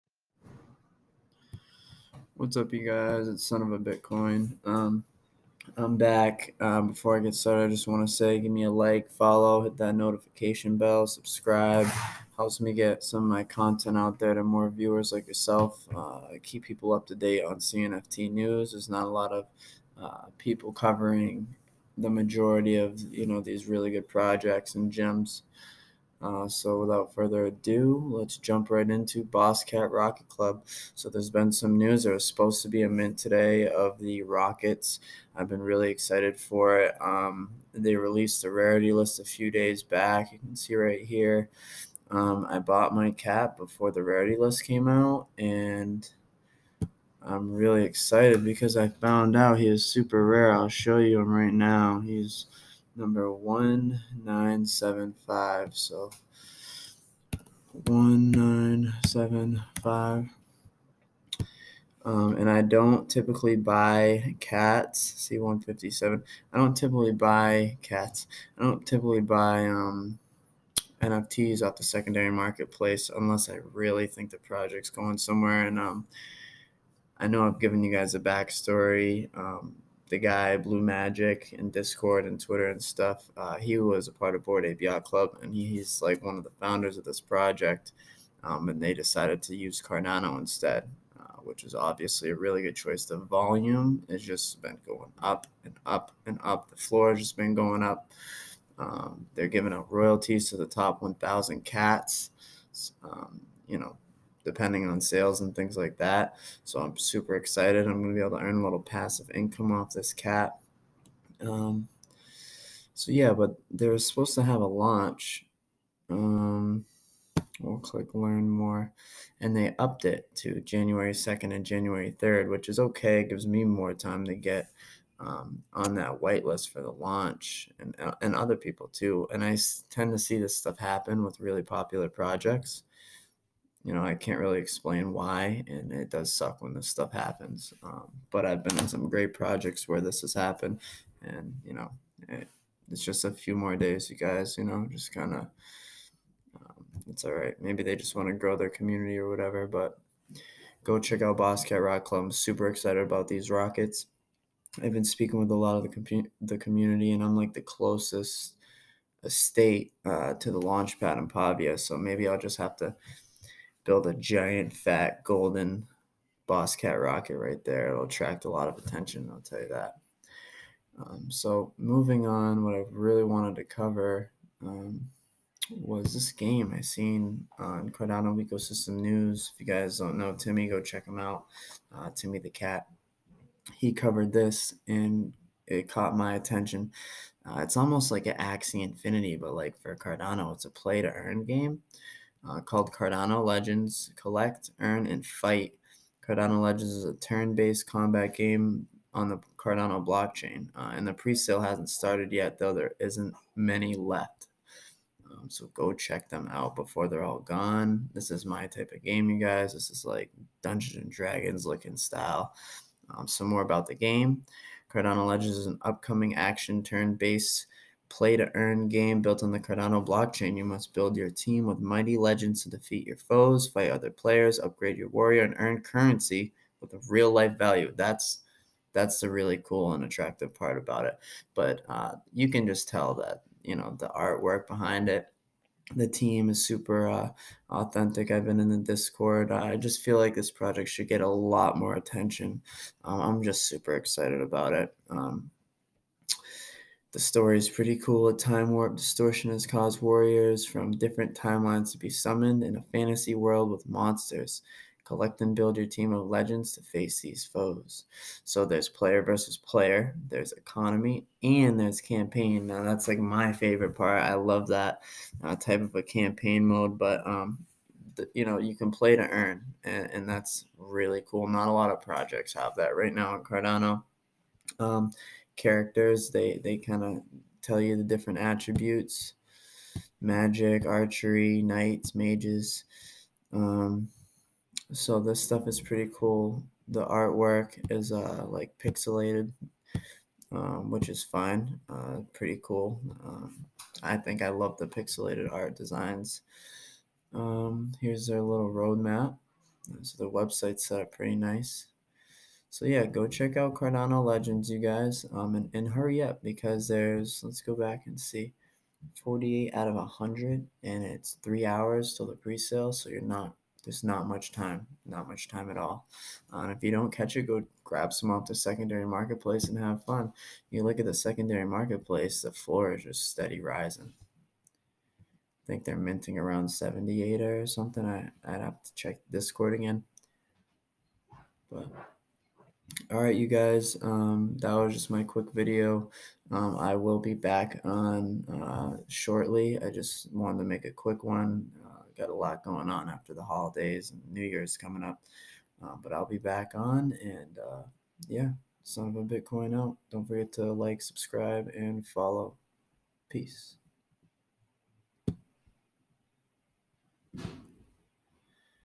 Cnft update- Boss cat rocket club, cardano legends new play to earn gem
minting is 33 ada fpr presale i made a mistake  in the clip but  this project looks very interesting! and ill be buying some cardano legends later today and very much in anticipation for the boss cat rocket club drop jan 2nd! boss af

heres a link to all of my platforms and merch!